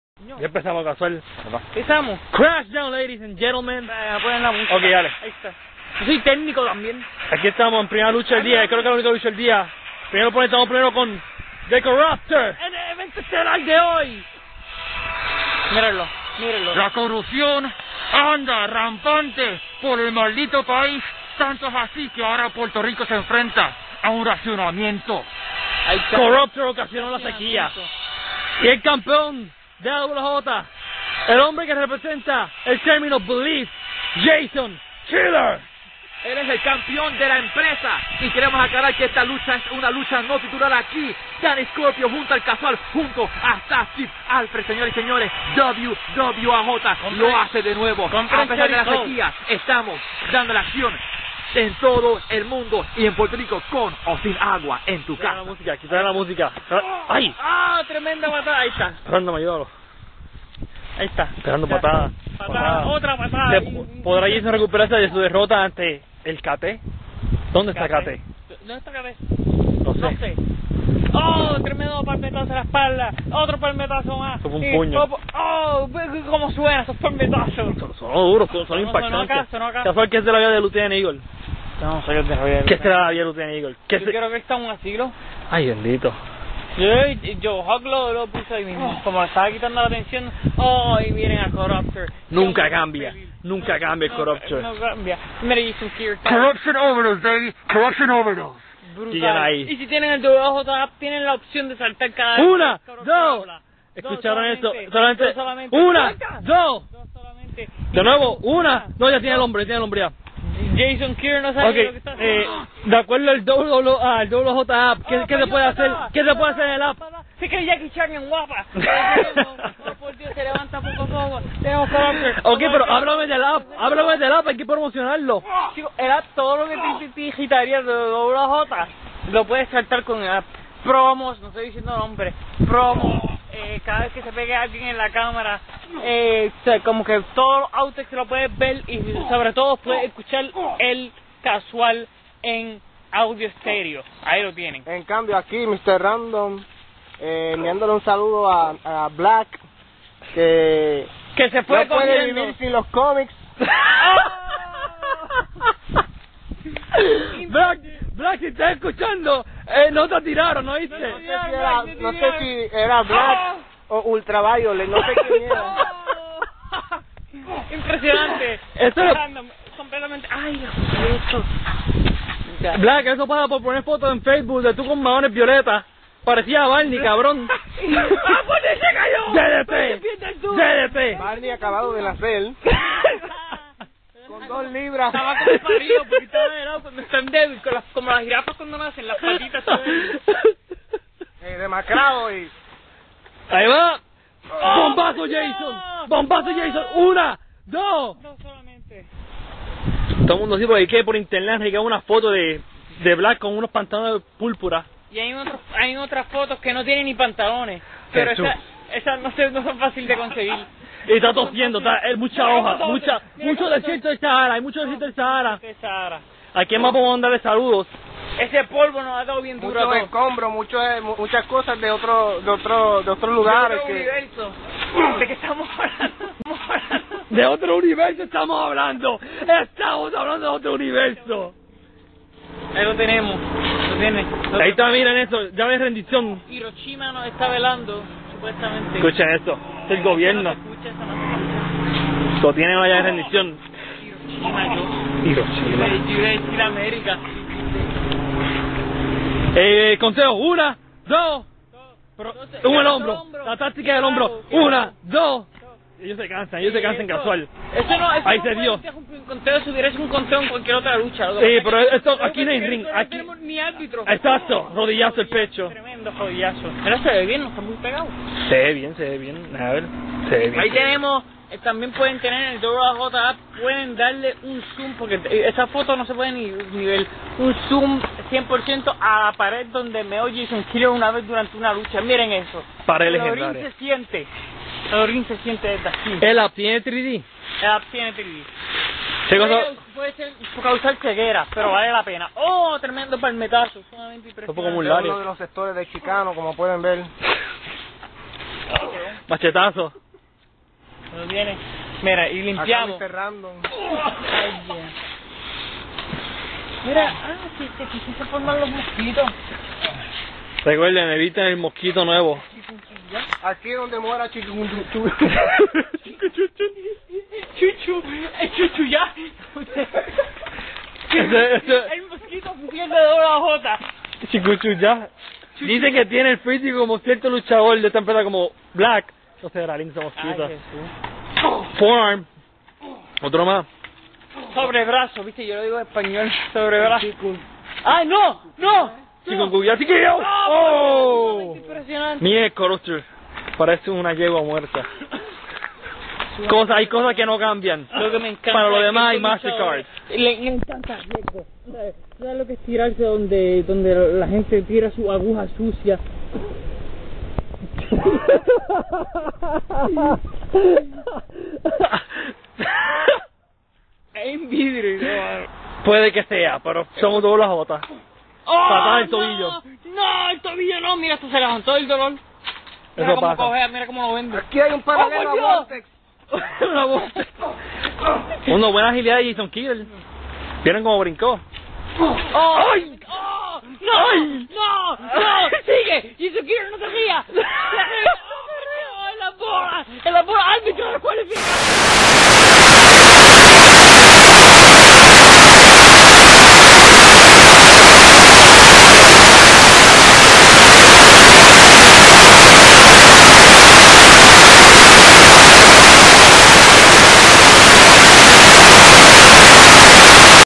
WWAJ- Singles match Backyard Wrestling
The Corruptor looks to CORRUPT WWAJ champ Jason Killer. Will Belief prevail over Corruption?